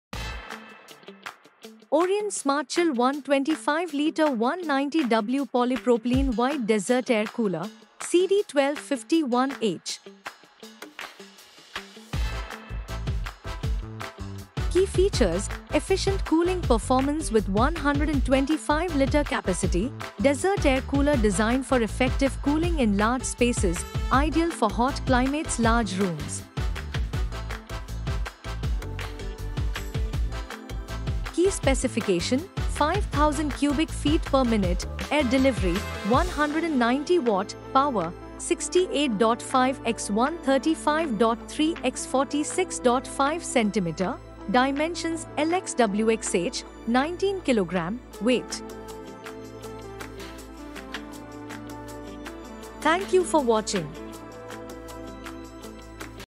Orient Smartchill 125 Litre 190W Air Cooler, CD1251H
Introducing the Orient Smartchill 125 Litre Desert Air Cooler, your ultimate solution for beating the heat in style. Designed to tackle scorching temperatures with ease, this cooler boasts a generous 125-liter capacity, ensuring efficient cooling performance for large spaces. Its desert air cooler design guarantees effective cooling, making it perfect for hot climates and sizable rooms. Say goodbye to sweaty discomfort and hello to refreshing coolness!